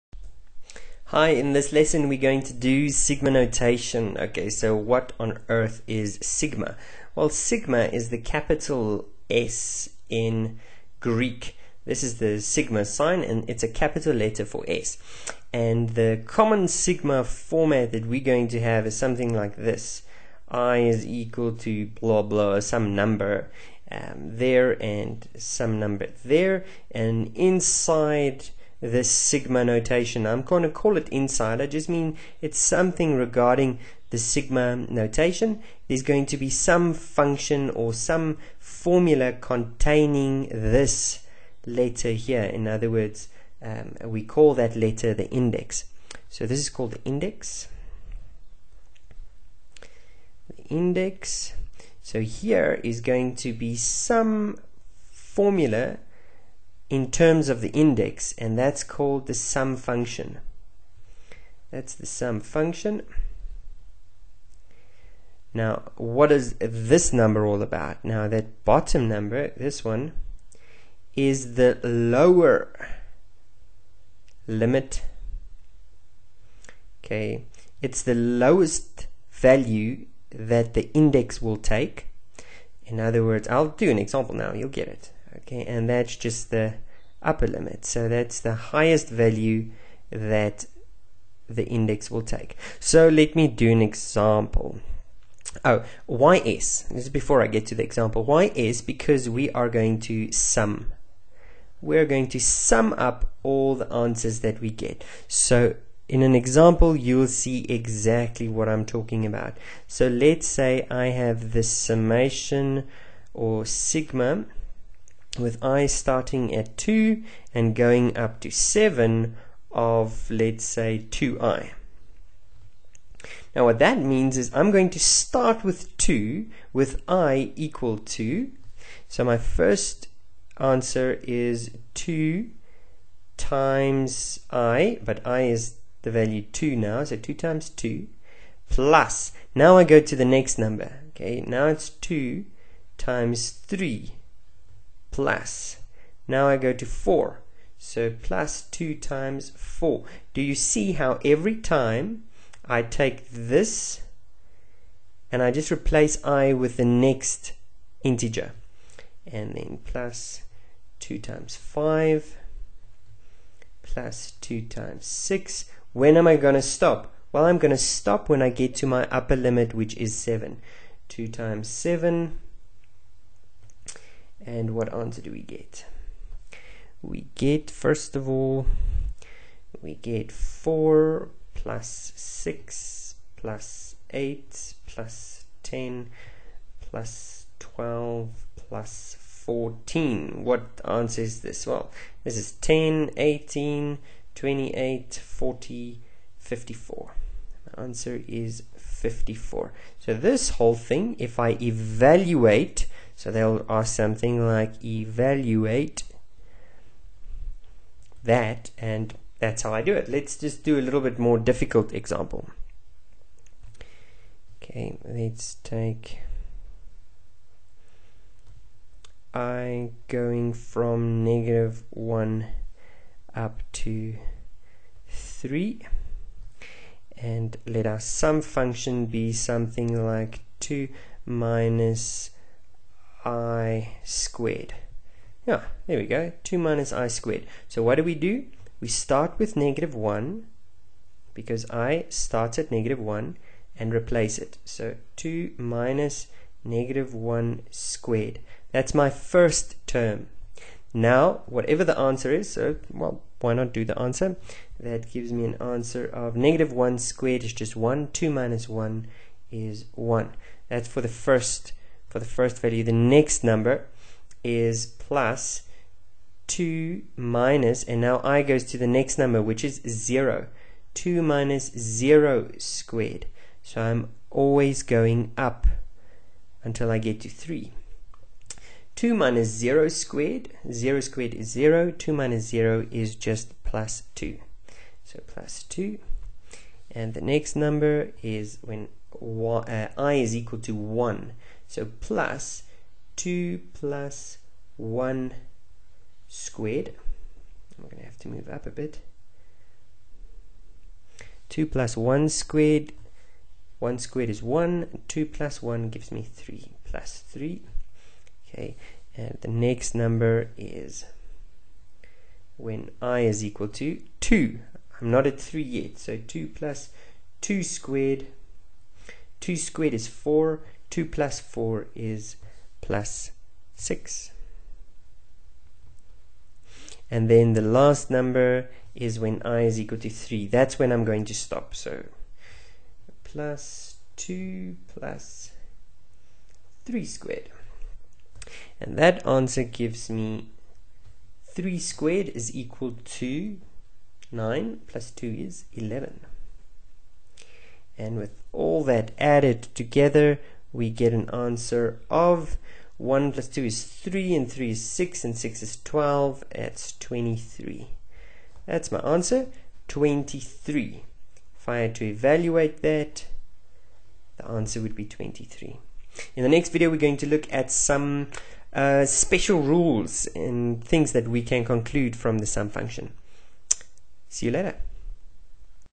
Sigma Notation
The basics of summation using sigma notation

For an online course that covers Sequences and series and other topics for Matric Mathematics join this course for free.